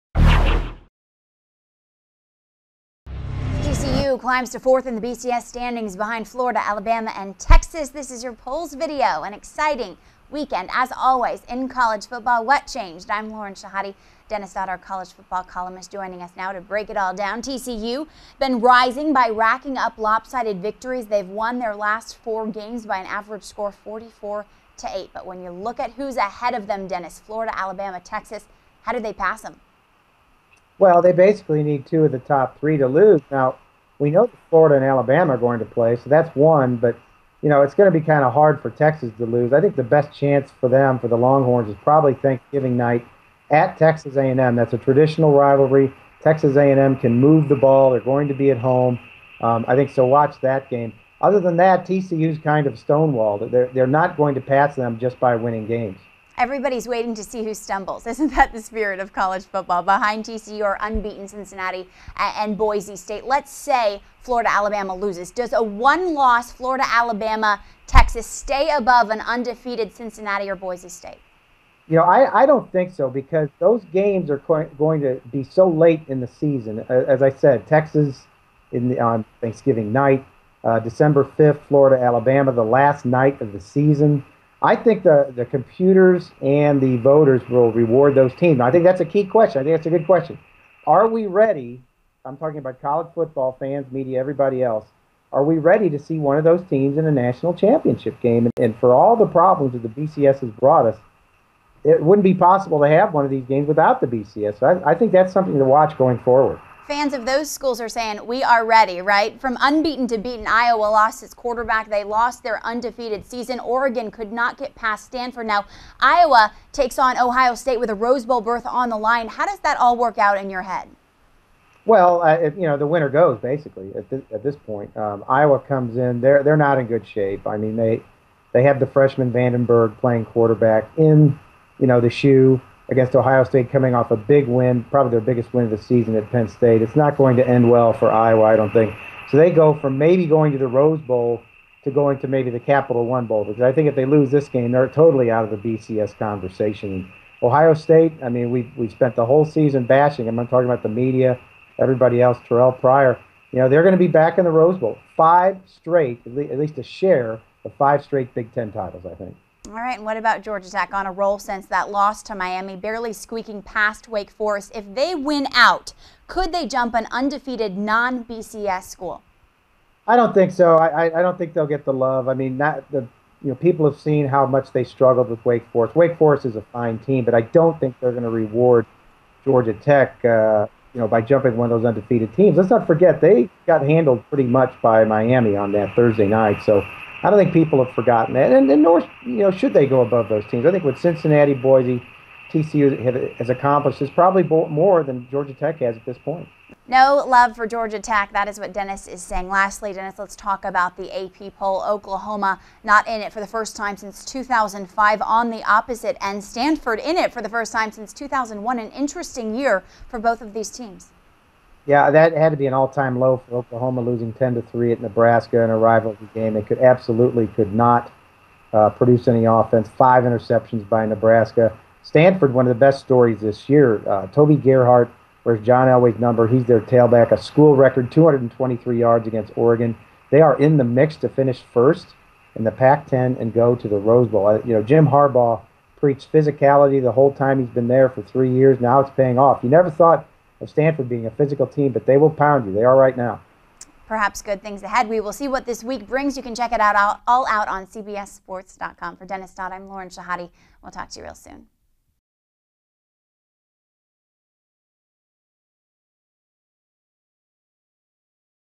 College Football Polls: Week 11
The TCU Horned Frogs are now fourth in the BCS Standings. Do they have a chance at reaching the National Championship game? Dennis Dodd joins Lauren Shehadi to discuss.